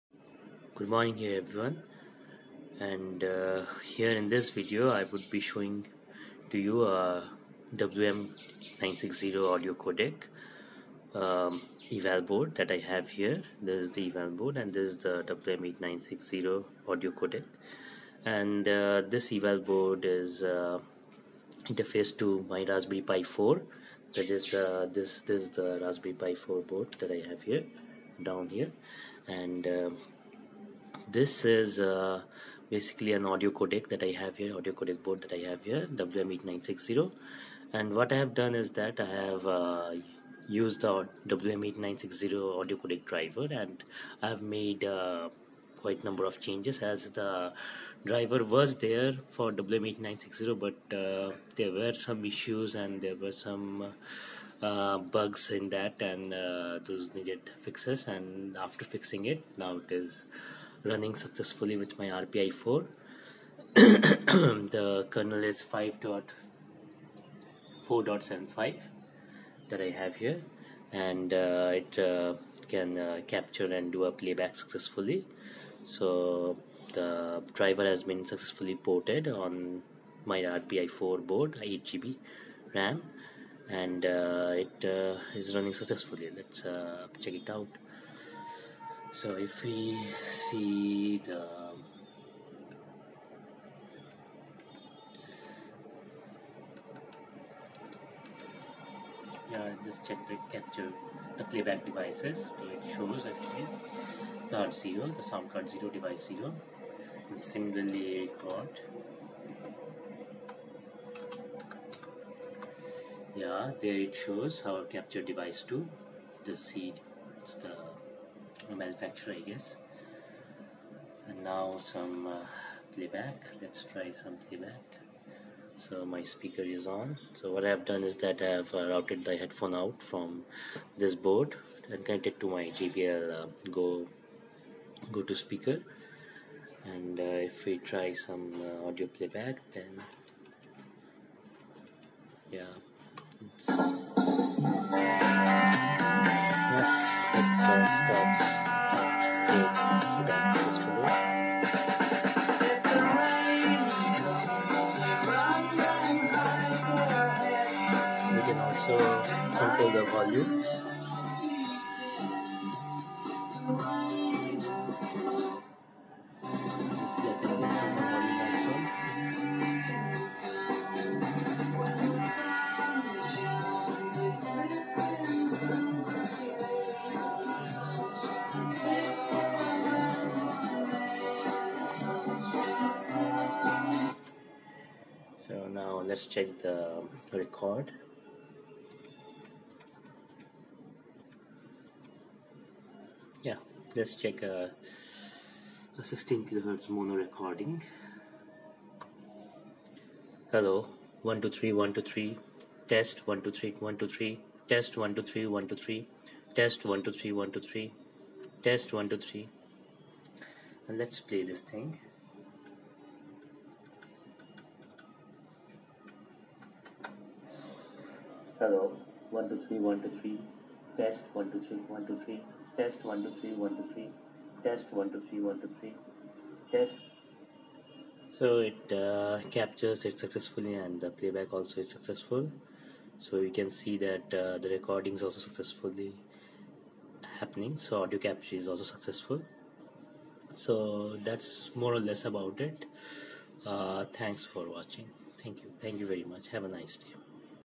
WM8960 Audio Codec with Rpi4
Here's my WM8960 Audio Codec (from Wolfson microelectronics) eval board interfaced with RPi4/8Gb on I2C. The Driver for WM8960 was having issues with RPi4 latest kernel, so required changes & fixes. Fixed the driver and now it works fine. Audio Playback using Headphone Out from WM8960 board to AUX-In of my JBL Go2 for 32-bit, 48kHz/44.1kHz, stereo works successfully. Also, Audio capture at 16kHz, Mono works great. Overall, good Audio codec with good support for noise reduction, ADCs, ALC(which is good for Speech capture), Soft mute for Anti pop noise suppression, High pass filters, Deemphasis et al...